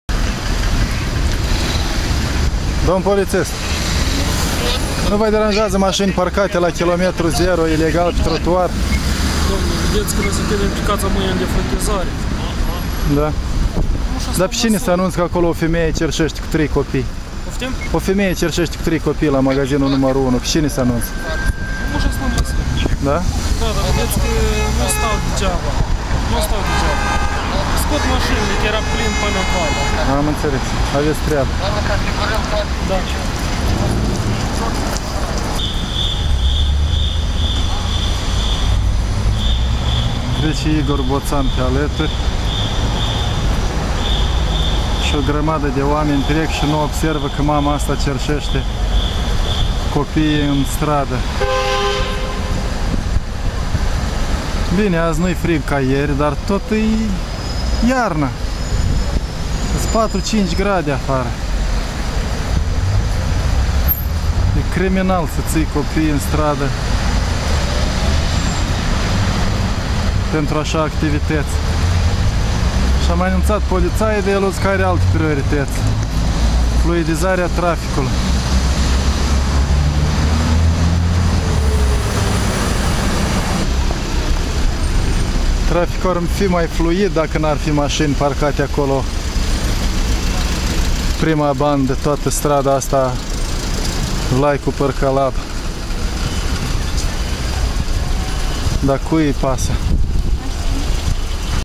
Cerşeşte cu 3 copii în stradă, poliţia are altă treabă - Curaj.TV
Chişinău, bulevardul central, kilometrul ziro al capitalei, peste drum de primărie